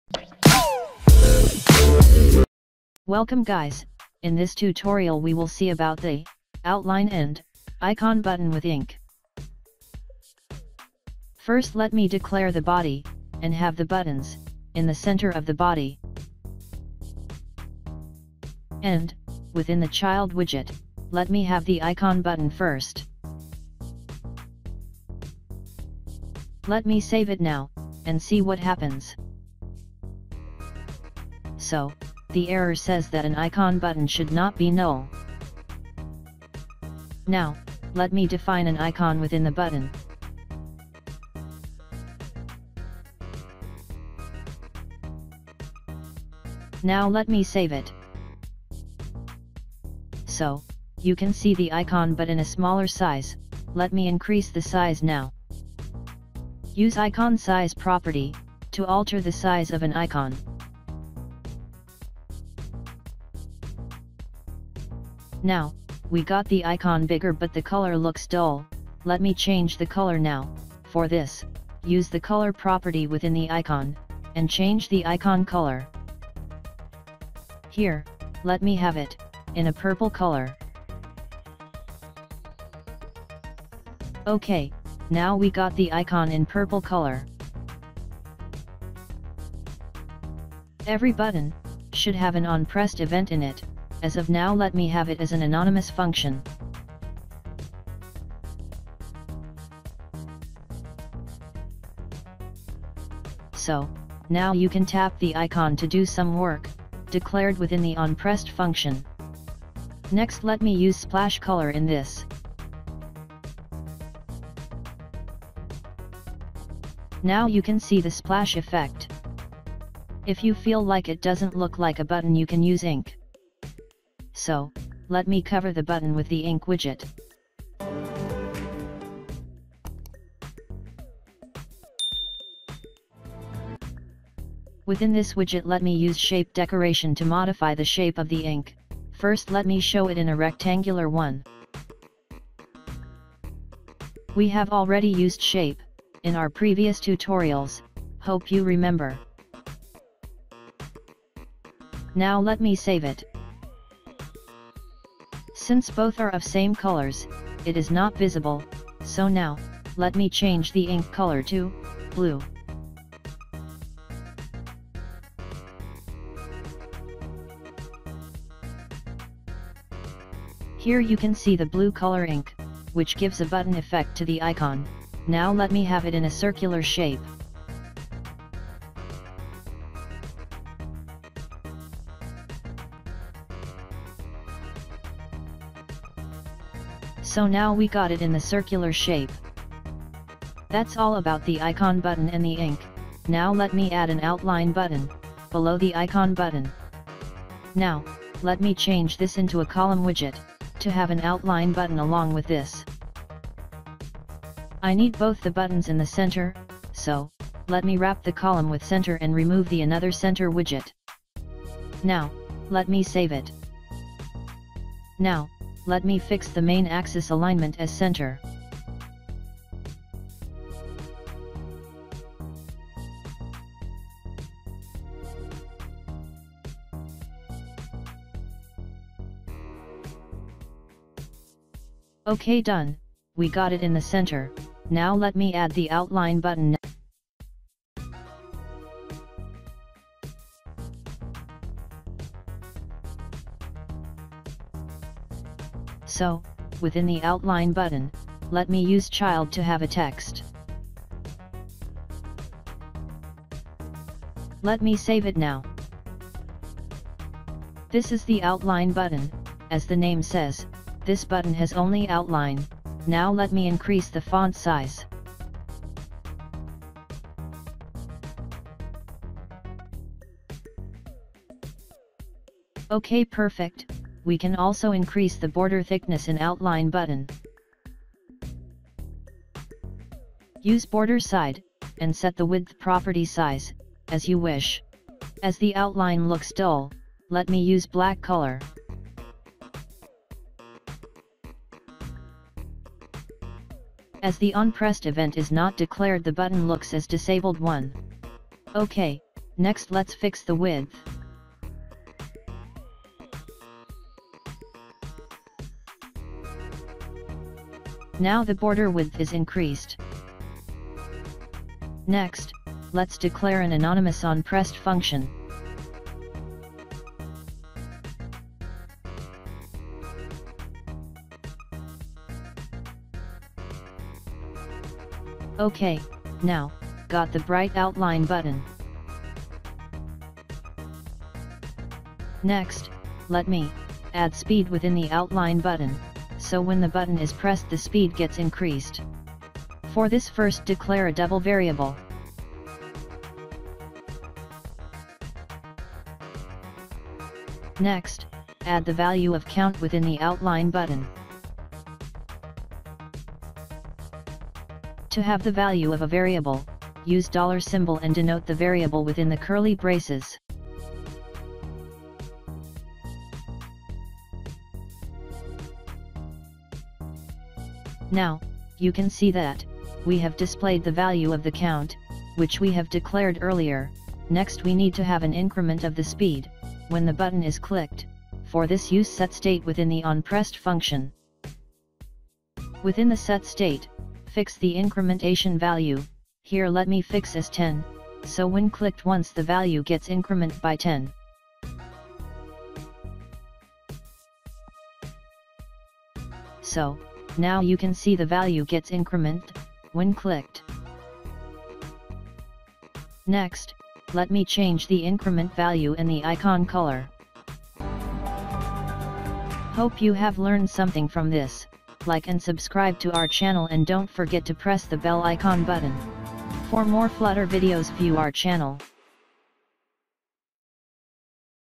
Outline button | Icon button with Ink | Flutter tutorial for beginners
Hey Guys,
            In this tutorial, we will see about the Outline and Icon buttons in detail, along with all its properties. This tutorial is mainly for beginners of flutter. Check out the flutter playlist for more videos of flutter. Hope you learned something from my tutorials.

We have already discussed the flat and raised buttons as a separate tutorial and floating action button(FAB) in our regular tutorial.

Topics covered:
           -- Outline button
           -- Icon button
           -- Ink